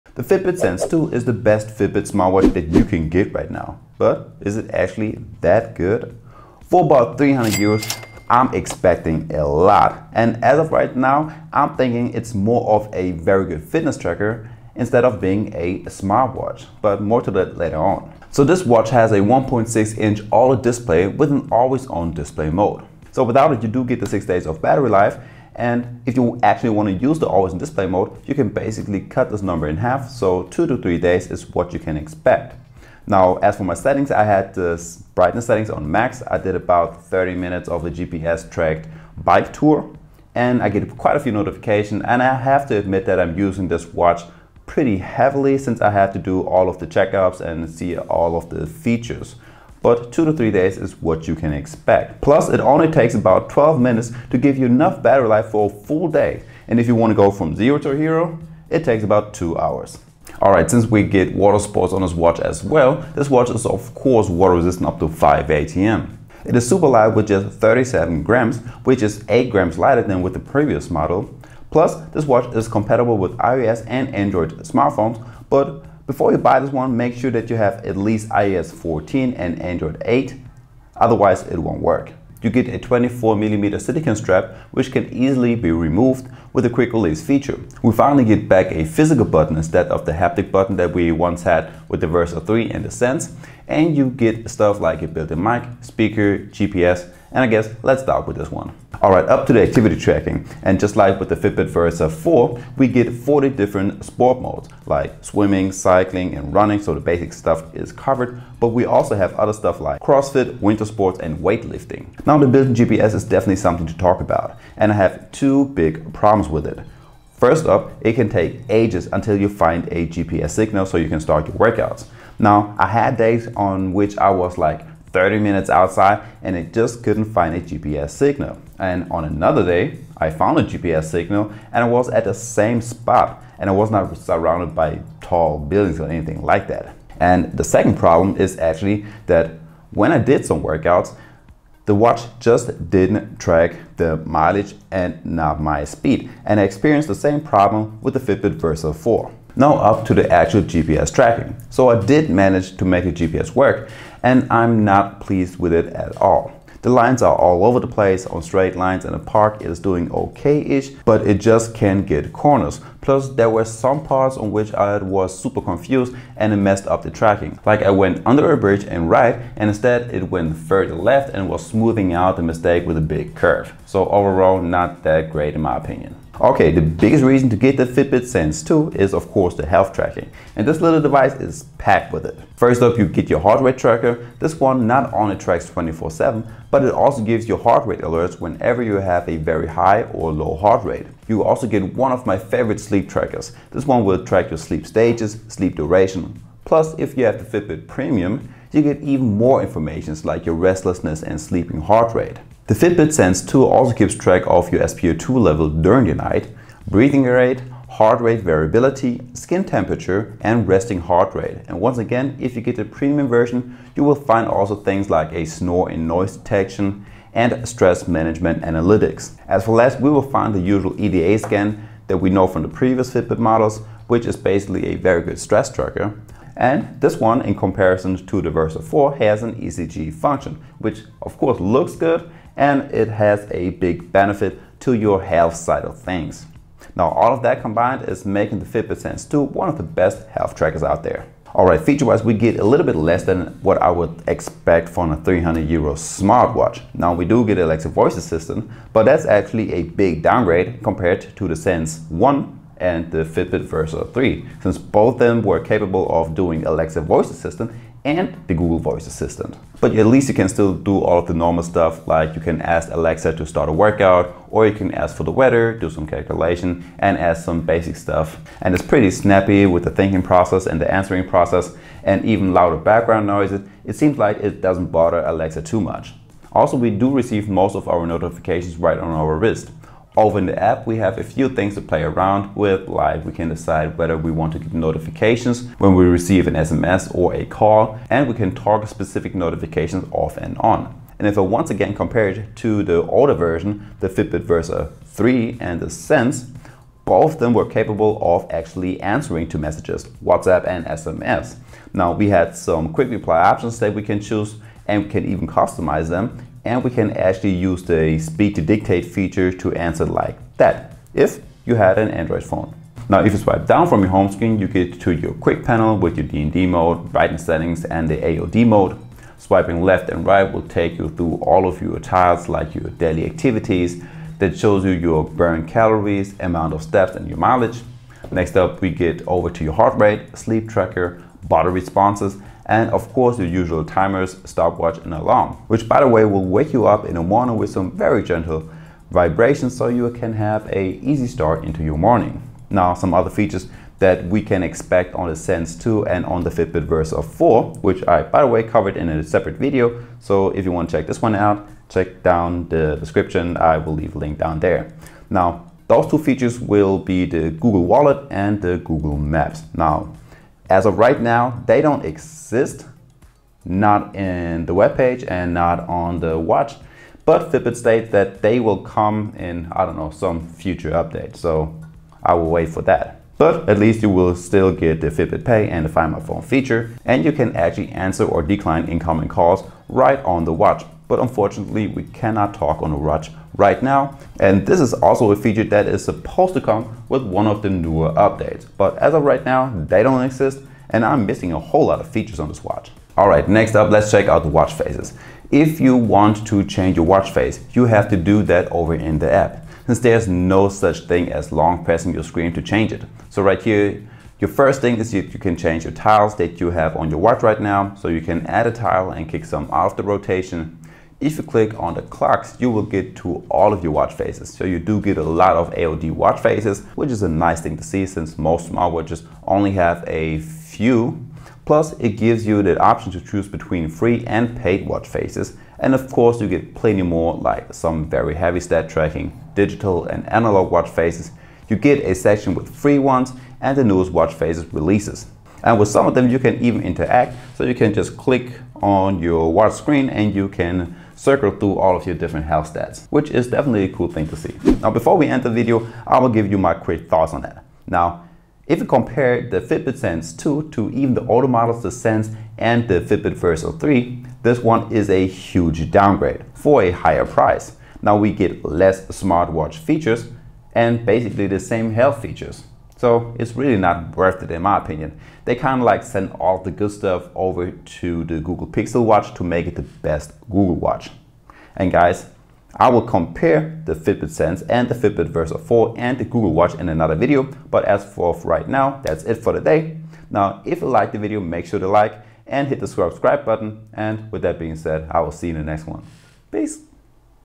Fitbit Sense 2 Review l Another Let DOWN! Why you should keep your hands of it...
Fitbit Sense 2 on Amazon
Fitbit Versa 4 on Amazon
Here we have the Fitbit Sense 2 and my personal review to it. but it's not as you expect it to be.

Time Stamps:
0:00 = Intro
0:18 = Specs & Battery Life
1:39 = Acitivity Tracking + GPS
2:55 = Health Features
4:01 = Smart Features
6:32 = Watch Faces
7:25 = My Thoughts